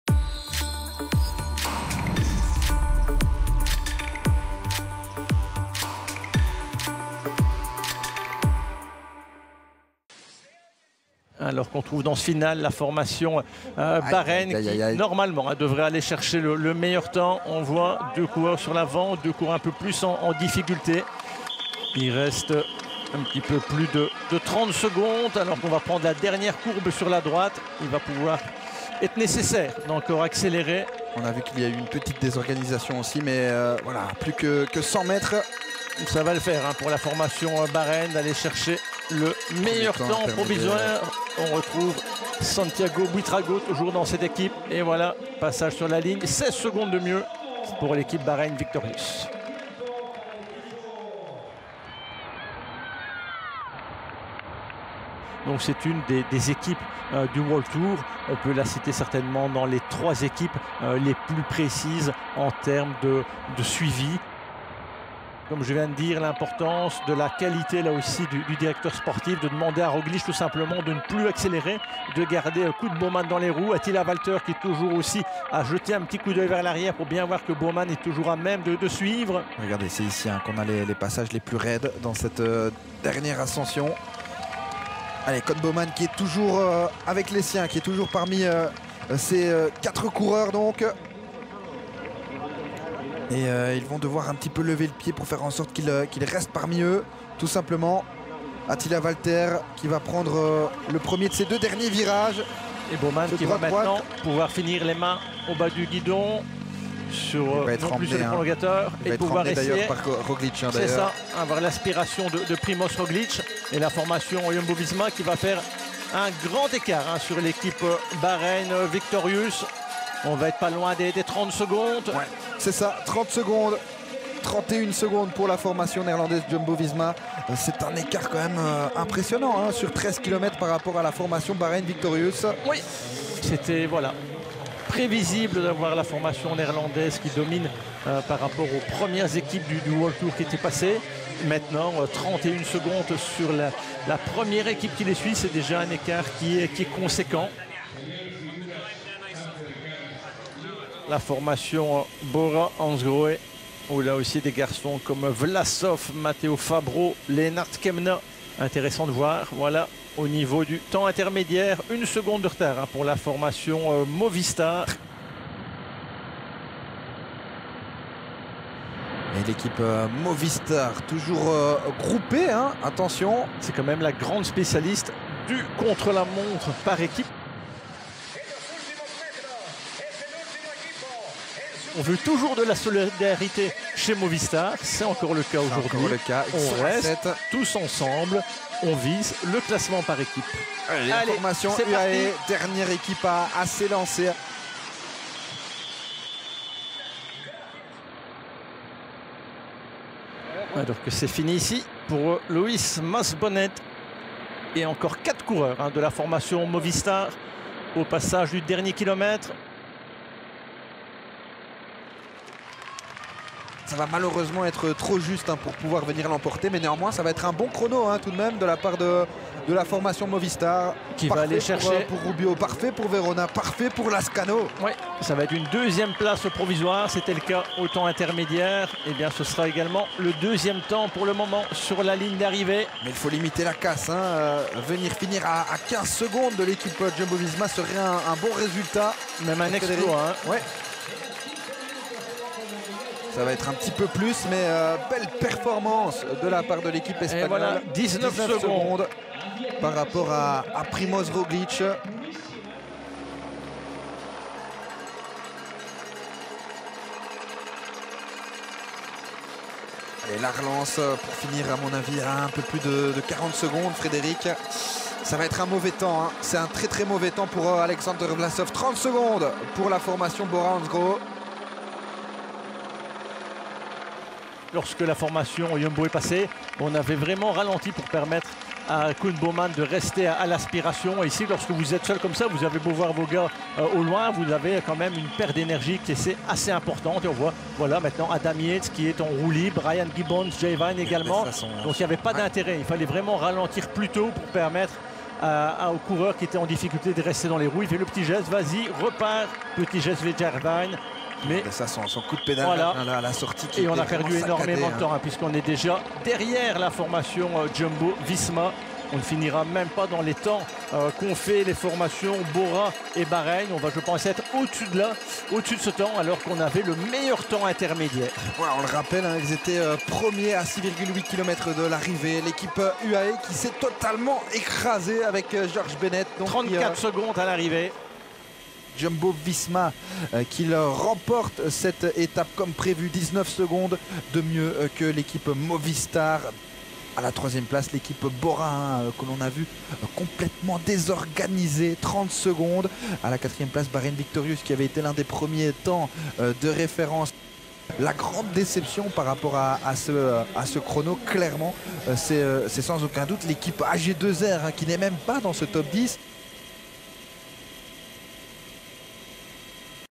Tour de Burgos 2023 | Résumé de la 2e étape
La deuxième étape de cette édition 2023 du Tour de Burgos était un contre-la-montre en équipe de 13 km.
Une étape importante pour le classement final de la semaine...